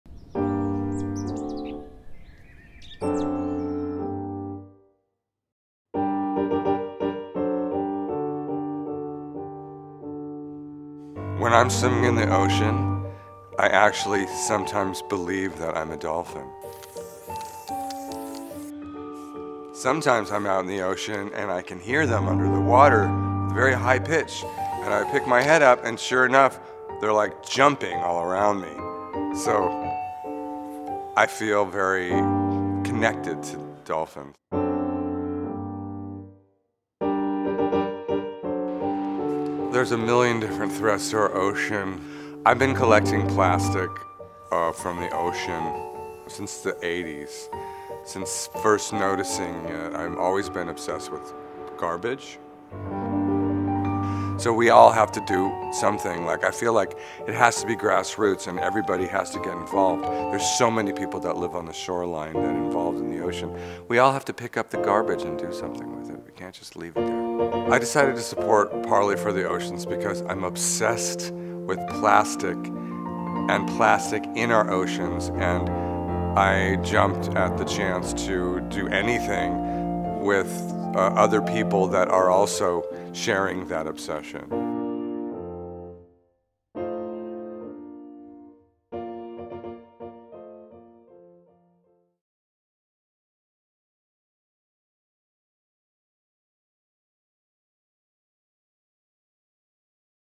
Kenny Scharf x Parley: Global Clean Up Network
Internationally acclaimed American artist Kenny Scharf launches together with Parley for the Oceans a series of individually painted surfboards to support Parley’s Global Clean Up Network with a special focus on Tulum, Mexico.